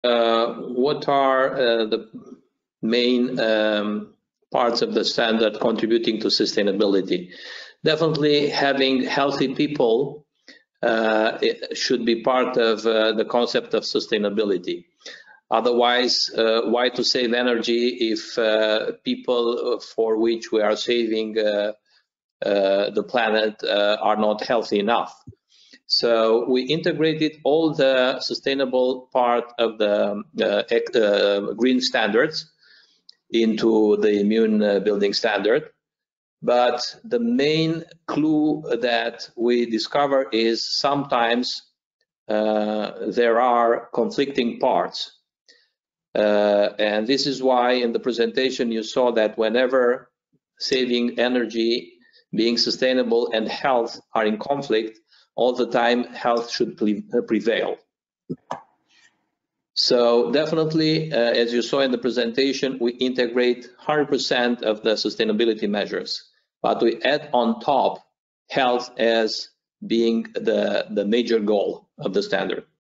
Liviu Tudor, Genesis Property President - CIOB Webinar about IMMUNE™- Sustainability of the standard
IMMUNE™ a fost conceput de Liviu Tudor, Președintele Federației Europene a Proprietății și Fondatorul și Președintele Genesis Property, alături de o echipă de experți și profesioniști, ca răspuns la amenințările împotriva sănătății, în timpul pandemiei de COVID-19. IMMUNE Building Standard™ (IMMUNE™), un standard global de sănătate creat pentru a certifica nivelul de imunitate al mediului construit, încorporează o rețea de dispozitive și tehnologii disponibile, echipamente specializate, personal dedicat și noi facilități, promovând reguli operaționale și de sănătate concentrate pe spațiul fizic în care interacționează ocupanții și vizitatorii.
Liviu Tudor a fost unul dintre invitații webinarului Chartered Institute of Building (CIOB) care s-a concentrat pe IMMUNE Building Standard™ - Primul standard de construcție open-source Healthy by Design din lume. În cadrul sesiunii de întrebări și răspunsuri, dr. Liviu Tudor a abordat tema aspectelor din IMMUNE Building Standard™ care contribuie la sustenabilitate și a explicat că sănătatea trebuie privită ca parte a acestui concept.
IMMUNE™ was engineered by Liviu Tudor, President of the European Property Federation and Founder and President of Genesis Property, alongside a team of experts and professionals as a response to the health threats, during the COVID-19 pandemic. The IMMUNE Building Standard™ (IMMUNE™), a global health standard designed to certify the built environment’s level of immunity, is based on a practical investment in a Healthy by Design System (HbDS), incorporating a network of devices and available technologies, specialized equipment, dedicated personnel, and new amenities, promoting operational and health rules focused on the physical space where occupants and visitors interact.
Liviu Tudor was one of the guests of the Chartered Institute of Building (CIOB) Webinar that focused on the IMMUNE Building Standard™ - World’s First Open-Source Healthy by Design Building Standard. During the Q&A session, Dr. Liviu Tudor addressed the issue of the IMMUNE Building Standard™ contributing to sustainability and explained that health should be viewed as part of this concept.